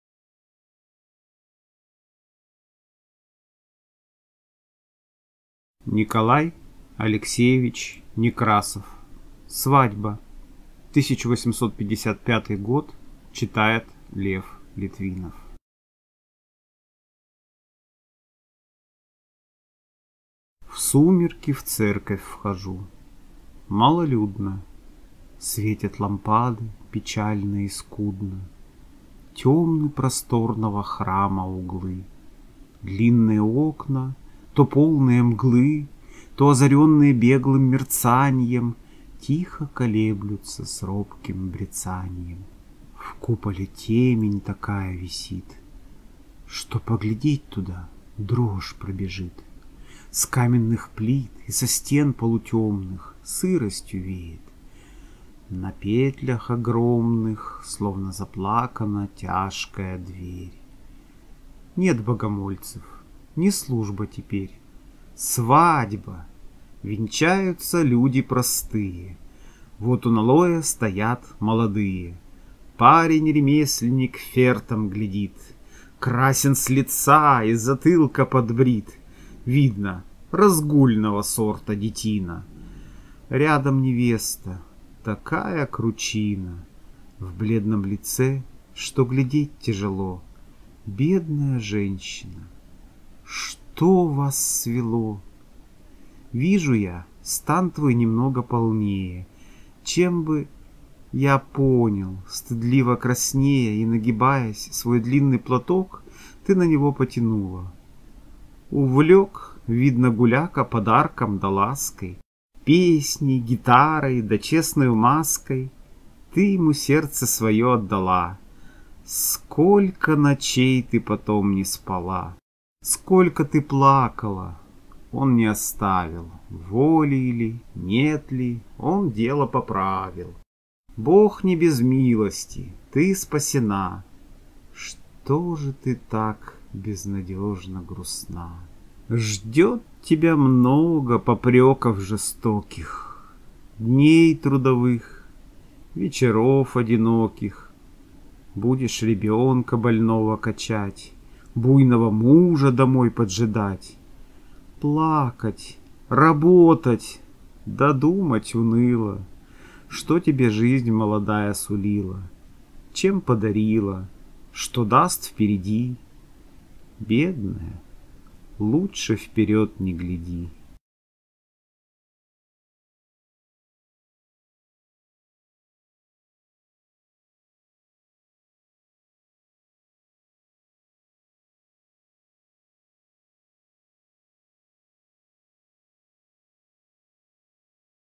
Некрасов Николай Свадьба Читает Лев Литвинов
Некрасов Николай Алексеевич.
Стих Свадьба.
Читает Лев Литвинов.
Слушать онлайн. Аудиокнига. Подкасты.
1855 год. Драма. Лирика.
Великие русские поэты.

В сумерки в церковь вхожу. Малолюдно,
Светят лампады печально и скудно,
Темны просторного храма углы;
Длинные окна, то полные мглы,
То озаренные беглым мерцаньем,
Тихо колеблются с робким бряцаньем.
В куполе темень такая висит,
Что поглядеть туда — дрожь пробежит!
С каменных плит и со стен полутемных
Сыростью веет: на петлях огромных
Словно заплакана тяжкая дверь…
Нет богомольцев, не служба теперь —
Свадьба. Венчаются люди простые.
Вот у налоя стоят молодые:
Парень-ремесленник фертом глядит,
Красен с лица и с затылка подбрит —
Видно: разгульного сорта детина!
Рядом невеста: такая кручина
В бледном лице, что глядеть тяжело…
Бедная женщина! Что вас свело?

Вижу я, стан твой немного полнее,
Чем бы… Я понял! Стыдливо краснея
И нагибаясь, свой длинный платок
Ты на него потянула… Увлек,
Видно, гуляка подарком да лаской,
Песней, гитарой, да честною маской?
Ты ему сердце свое отдала…
Сколько ночей ты потом не спала!
Сколько ты плакала!.. Он не оставил,
Волей ли, нет ли, он дело поправил —
Бог не без милости — ты спасена…
Что же ты так безнадежно грустна?

Ждет тебя много попреков жестоких,
Дней трудовых, вечеров одиноких:
Будешь ребенка больного качать,
Буйного мужа домой поджидать,
Плакать, работать — да думать уныло,
Что тебе жизнь молодая сулила,
Чем подарила, что даст впереди…
Бедная! лучше вперед не гляди!